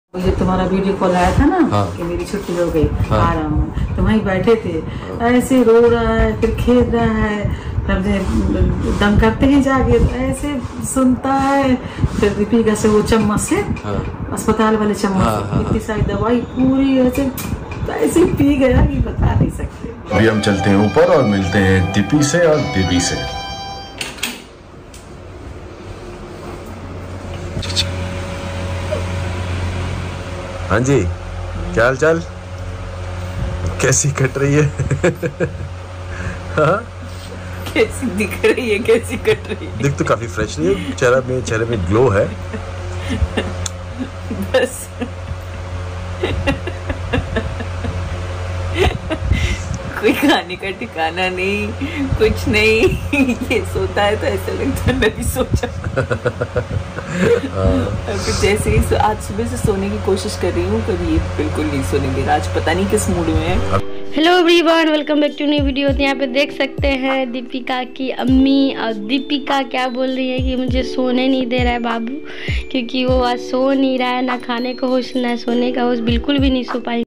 Baby Bilkul -V Sone Nhi Deta Hai Dipika Ko Ammi Ne Kay Kaha 🥰 Shoaib Ibrahim Vlog
Baby Bilkul -V Sone Nhi Deta Hai Dipika Ko Ammi Ne Kay Kaha 🥰 Shoaib Ibrahim Vlog Dipika ki duniya